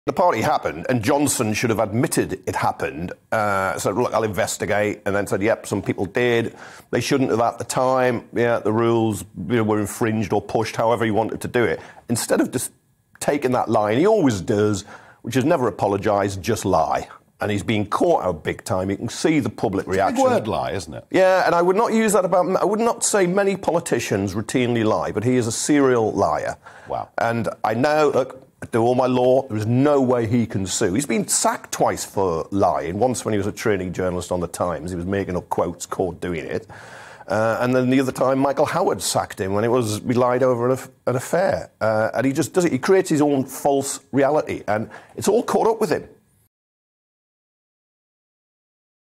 Kevin Maguire says Boris should admit that there was a 'Christmas party in Downing Street'
‘I would not say many politicians routinely lie but he is a serial liar’

Associate Editor at the Daily Mirror Kevin Maguire says Boris should admit that there was a Christmas party in Downing Street that broke Covid restrictions.